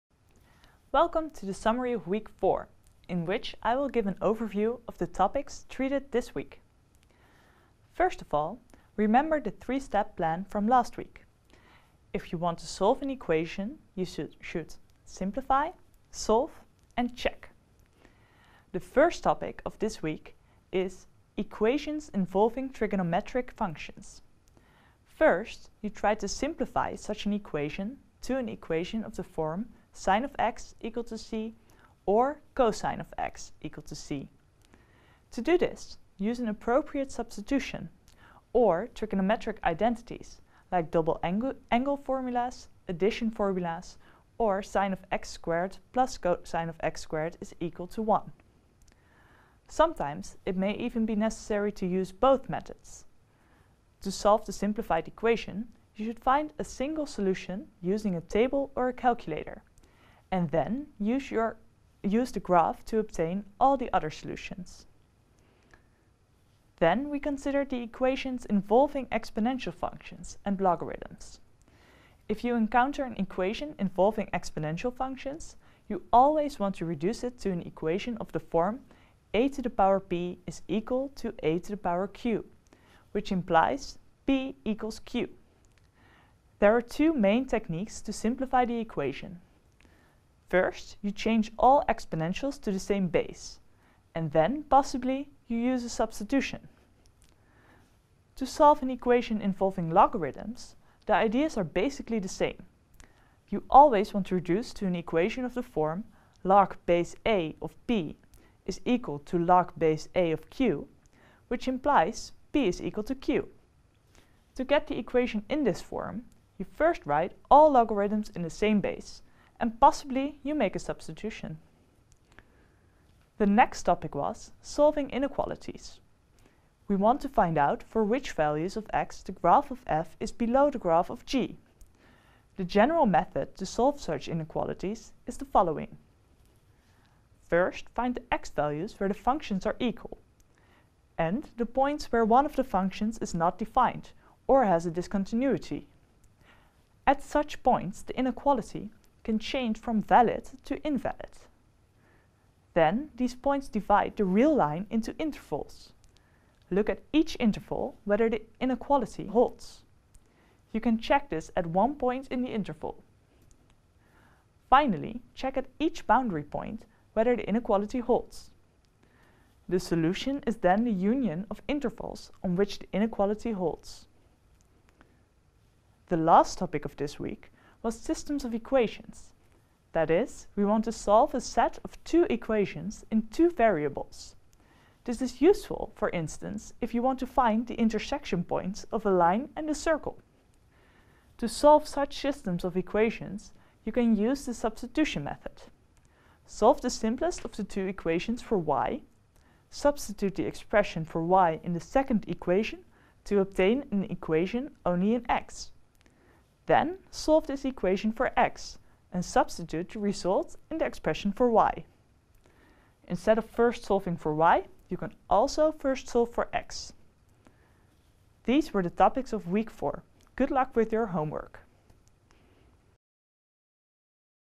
Summary solving (in)equalities - Equations - Mathematics - Pre-university Calculus - TU Delft
This educational video is part of the course Pre-University Calculus, available for free ©️ TU Delft, released under a CC BY NC SA license How do you solve trigonometric equations (sine, cosine) and equations involving exponentials and logarithms? How do you solve an inequality or a system of equations? This video gives the summary of week 4 of the edX course: pre-university Calculus (Calc001x)